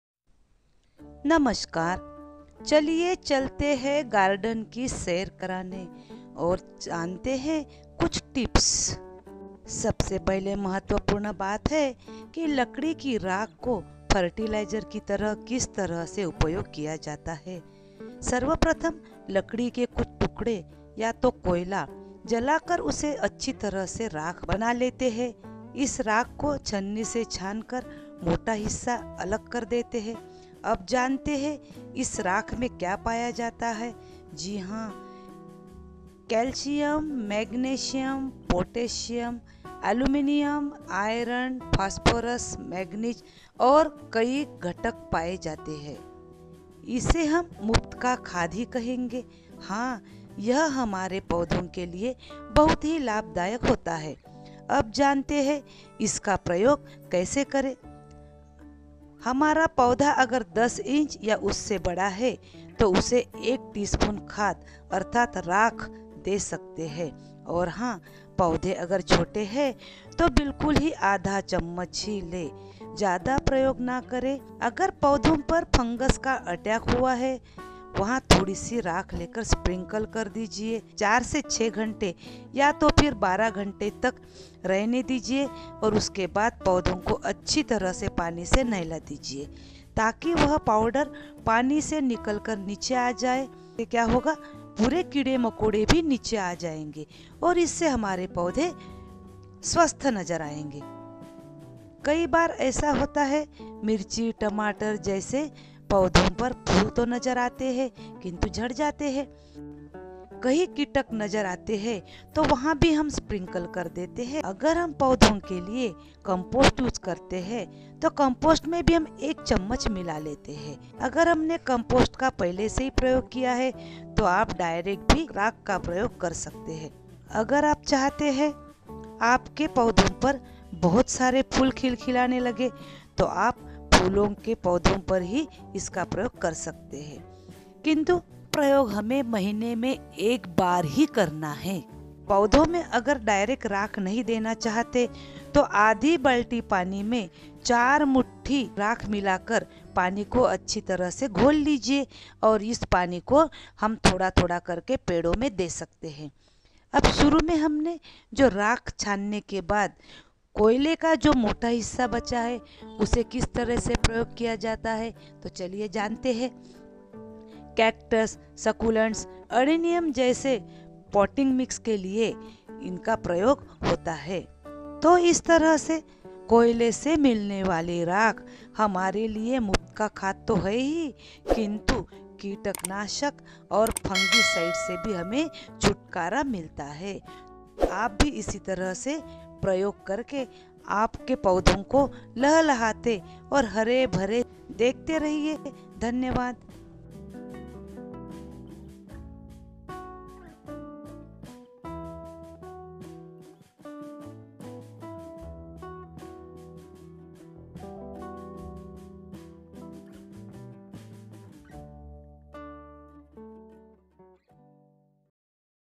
पौधो में राख का प्रयोग करे और पौधोंको स्वस्थ बनाये रखे, मूफ्त का खाद तो मिलेंगा ही,
कीटक तथा फंगीसाईड से छुटकारा
How to use wood ash
Uses of wood ash on plants
Charcoal ash for your plants